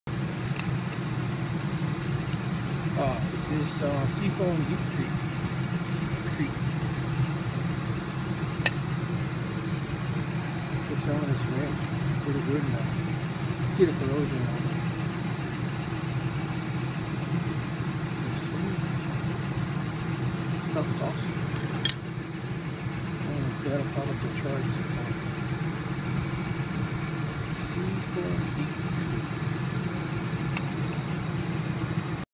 Seafoam Deep Creep Oil
Tried out Deep Creep for the first time. Used this rusted craftsman wrench. I was amazed how well it worked. I didn't think it would loosen this wrench.  Definitely a product I will be rebuying.
It had no bad odors to it. And the price was right. And the wrench spun smoother than it was new.